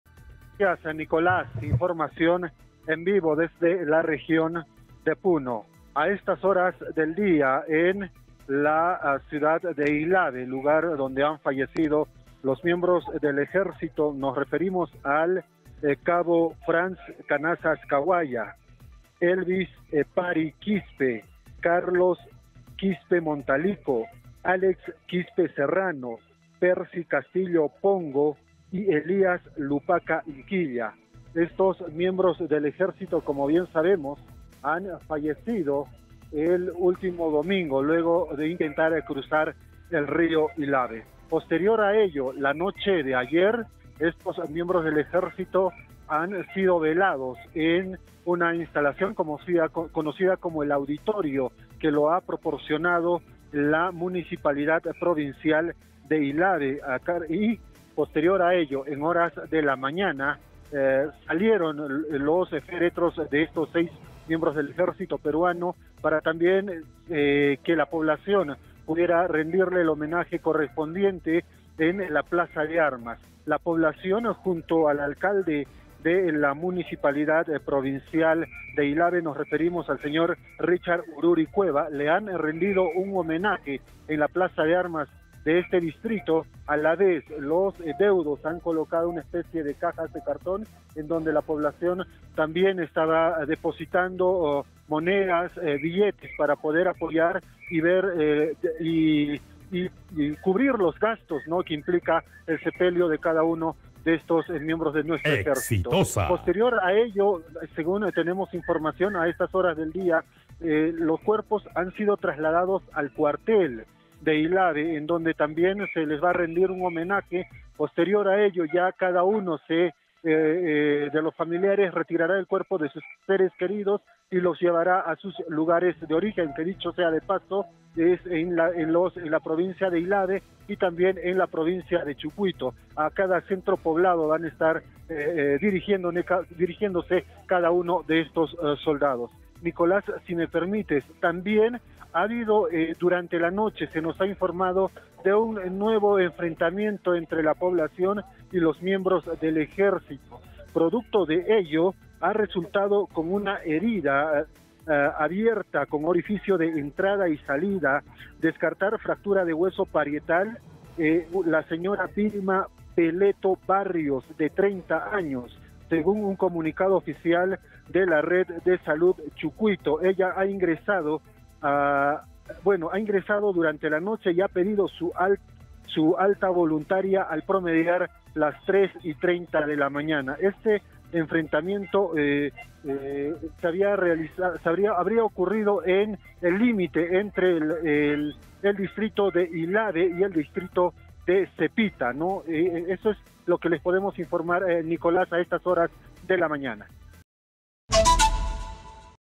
🔴🔵Puno: Población aymara se despide de militares en la plaza de Ilave
En Puno, pobladores aymaras se despiden de militares que murieron tras intentar cruzar río Ilave.

Exitosa Perú
"Exitosa: La voz que integra al Perú"

Corporación Universal